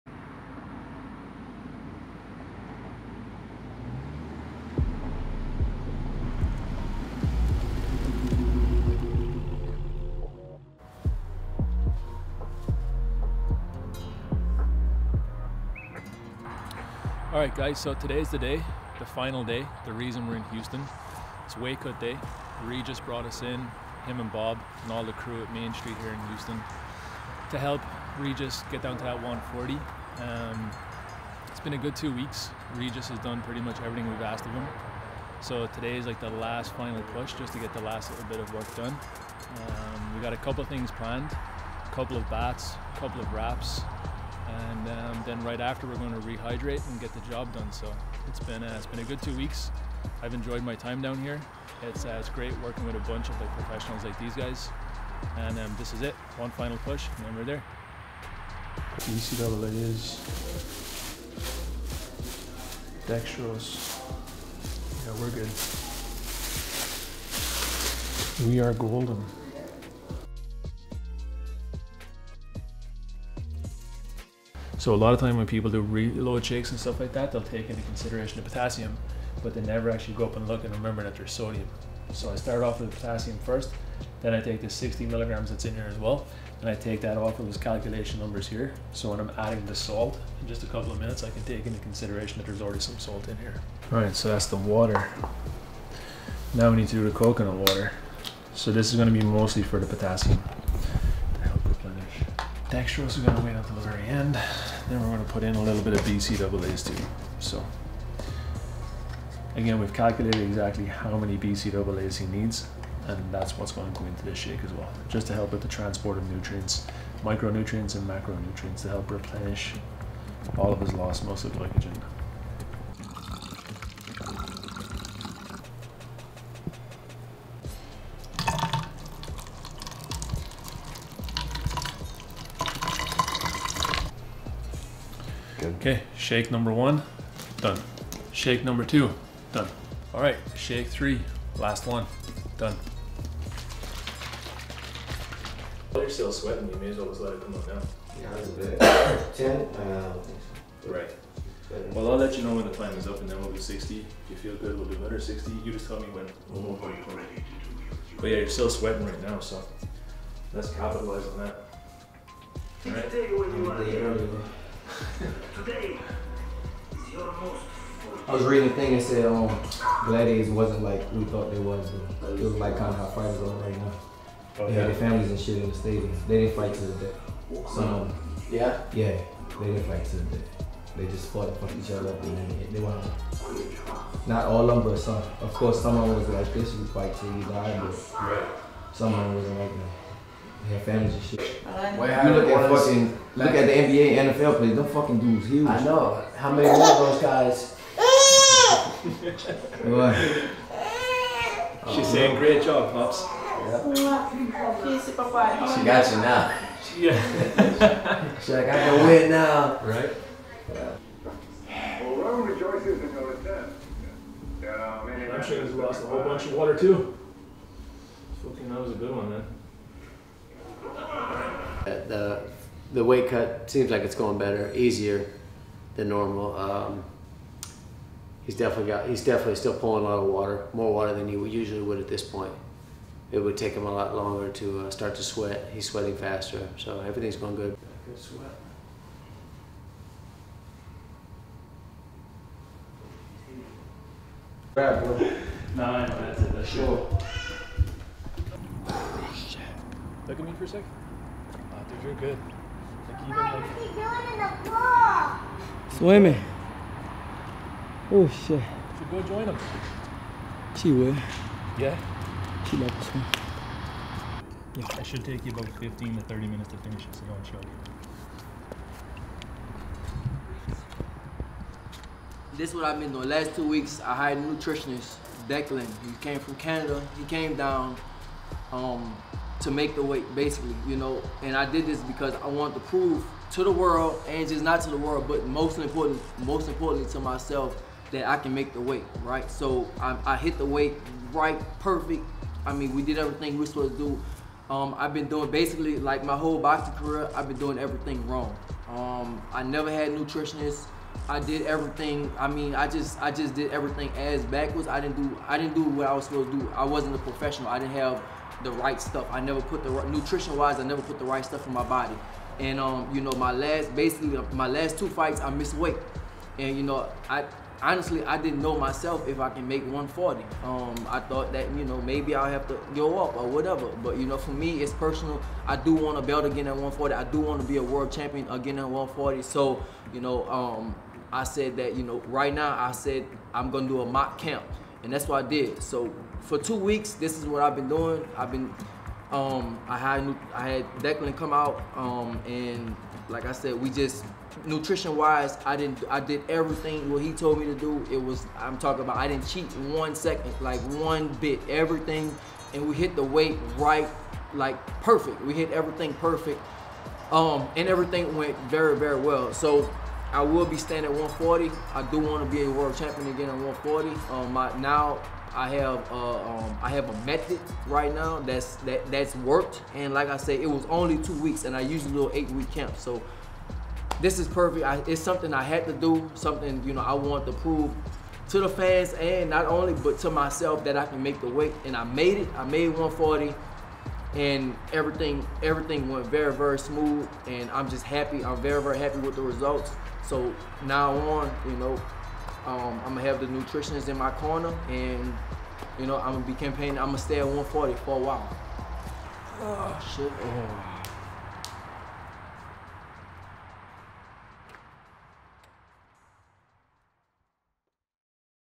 Regis Prograis - Weight Cut & Rehydration Recap
This is a short recap video of my time in Houston, Texas, working with 140lb World Champion, Regis Prograis.

This is a short recap video of our last couple of days in Houston, Texas, working with 140lb World Champion, Regis Prograis.

2022 is gonna be a huge year for Regis & all of the team. I am honored to be apart of this journey to becoming Undisputed 140lb World Champion.